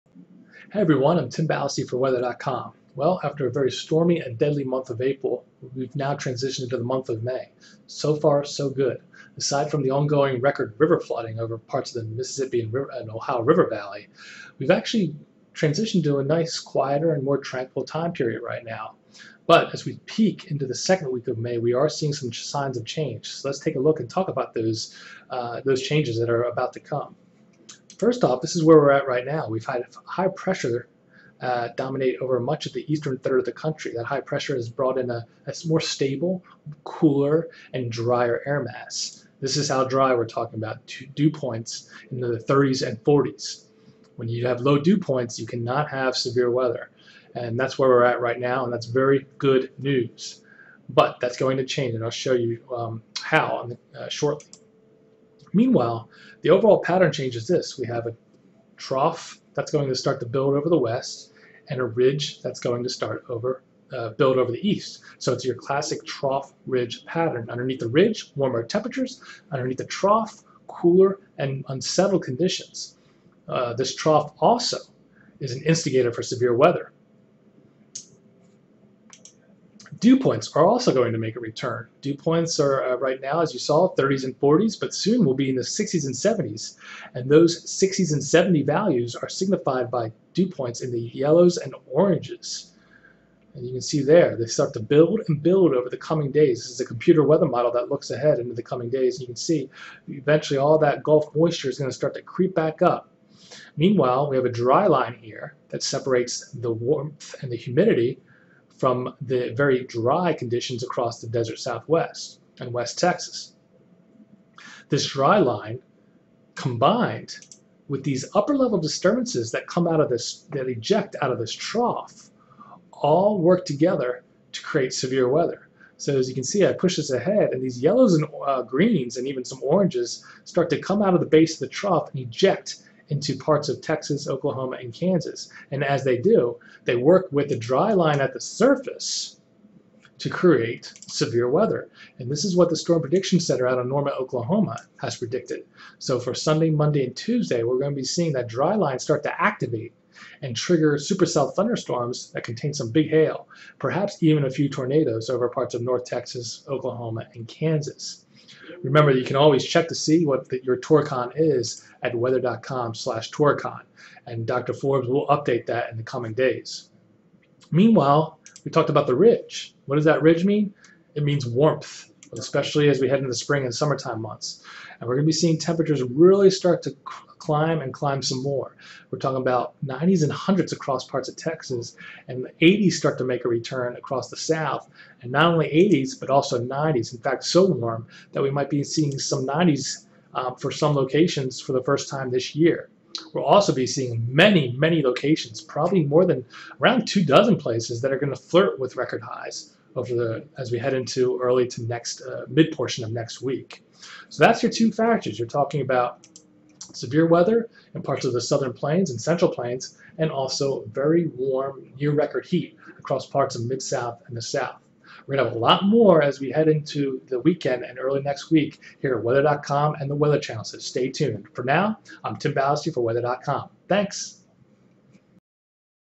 A Pattern Change is Coming for the Second Week of May 2011
A pattern change (trough - ridge) is coming for the 2nd week of May 2011 which will spell out two changes for parts of the United States - record heat and the return of severe weather.